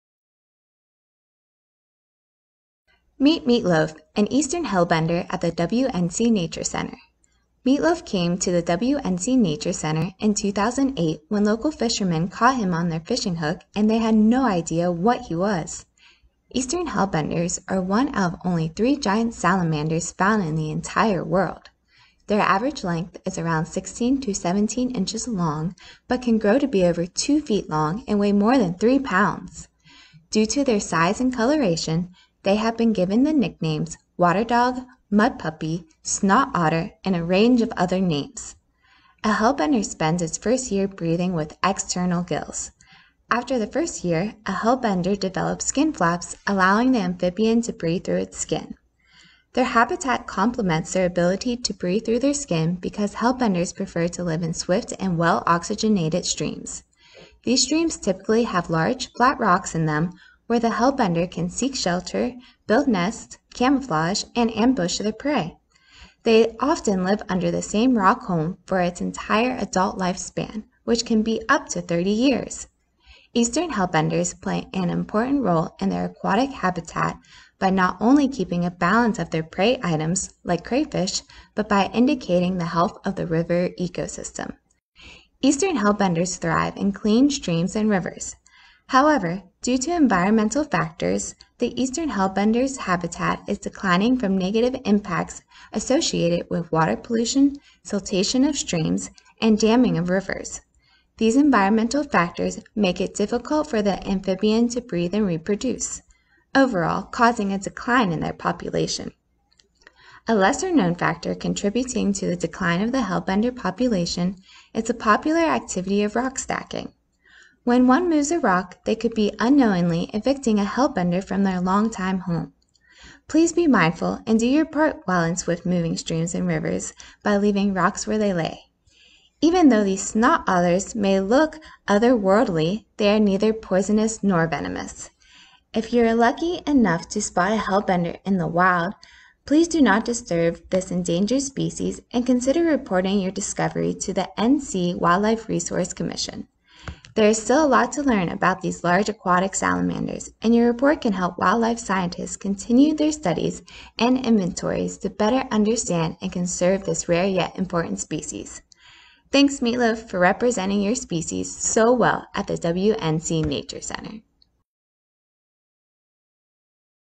Meet Meatloaf, the hellbender!
Here's your chance to learn a little more about some of the special animals who call the WNC Nature Center home! You may already know Meatloaf's favorite dish and favorite singer, but here are some lesser known facts about our Eastern hellbender!